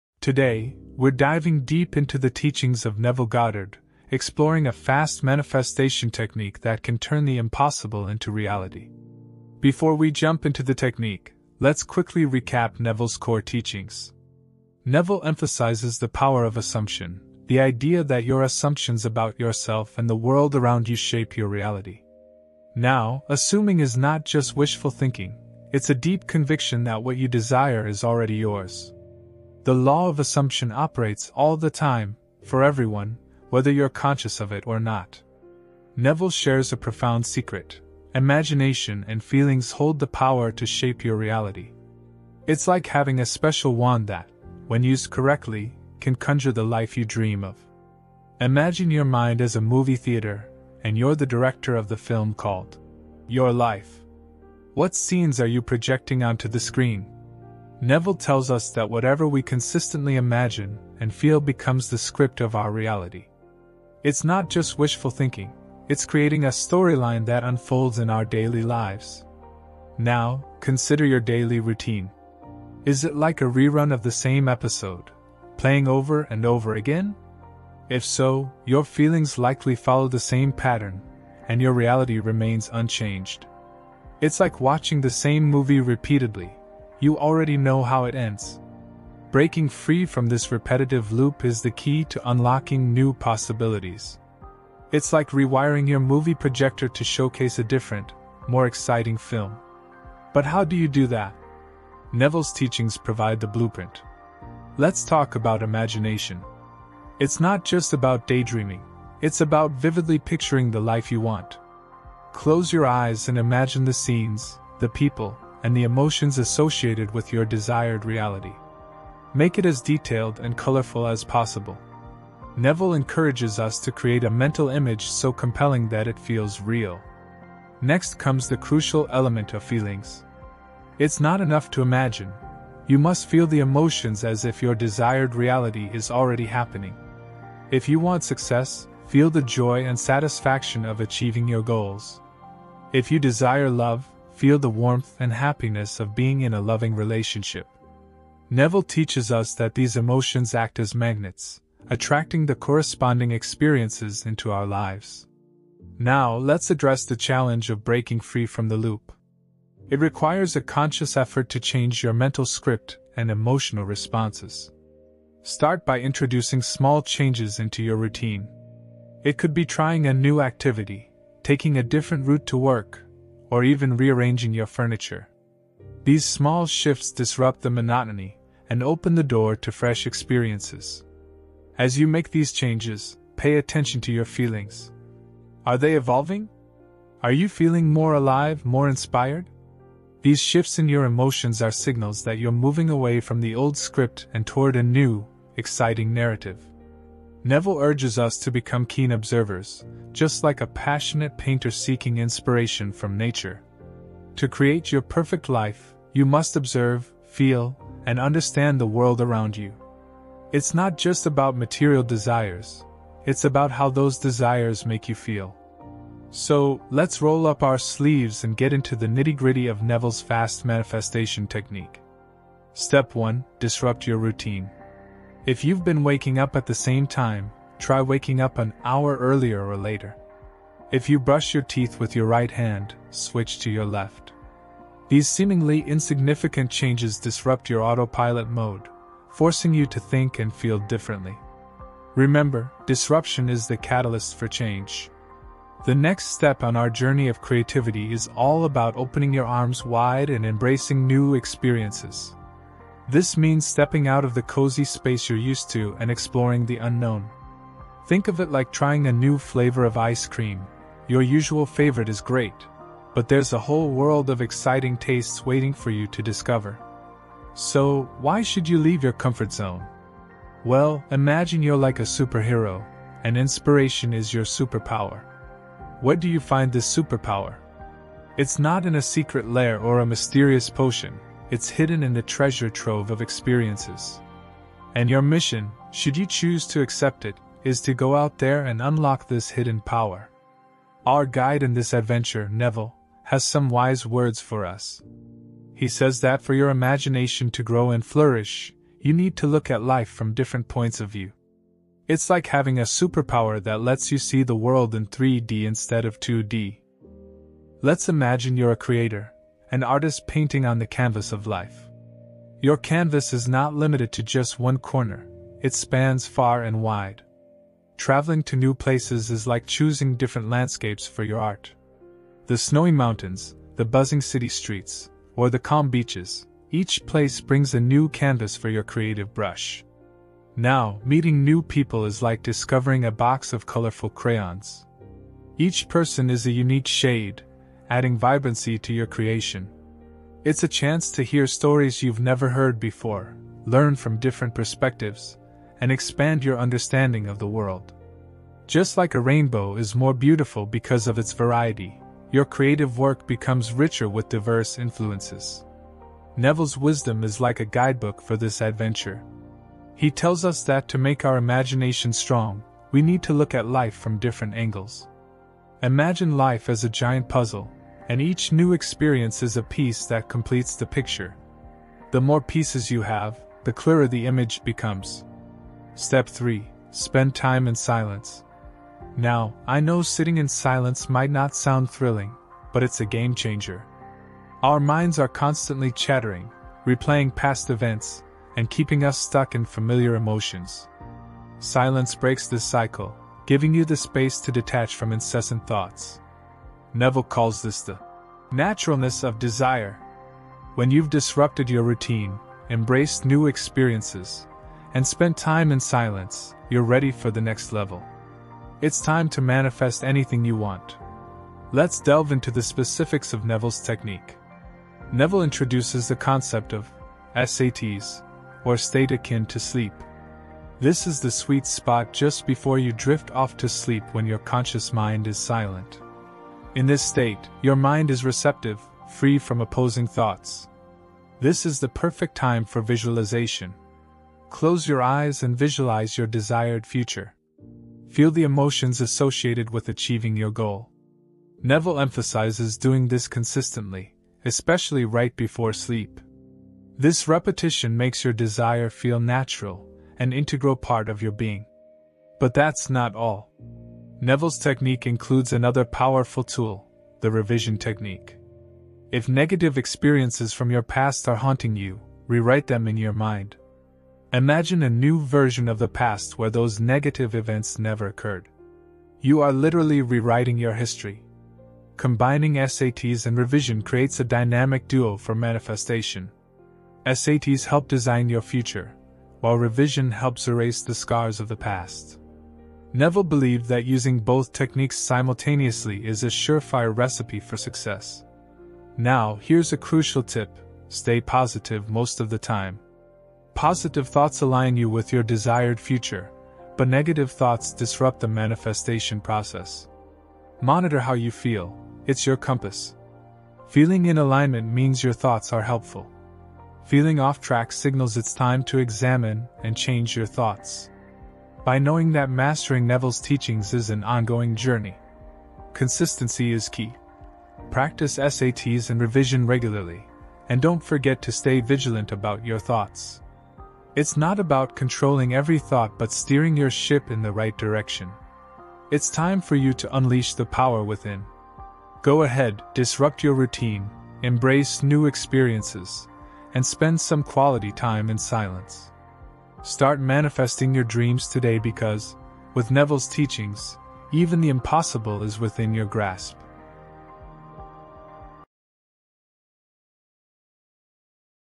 DO THIS FAST! Even The Impossible Will Manifest - Neville Goddard - Law Of Assumption
Unlock the power of manifestation with Neville Goddard's transformative teachings in this captivating video.  This content is tailor-made for individuals seeking to manifest their objectives effortlessly. Discover the simple and uncomplicated manifestation process that Neville Goddard advocates, and delve into the profound concepts and techniques that have taken the Law of Attraction to new heights. Explore the Law of Assumption and its pivotal role in bringing your desires to fruition. Get ready to unlock your full potential and manifest the life you've always dreamed of with Neville Goddard's timeless wisdom.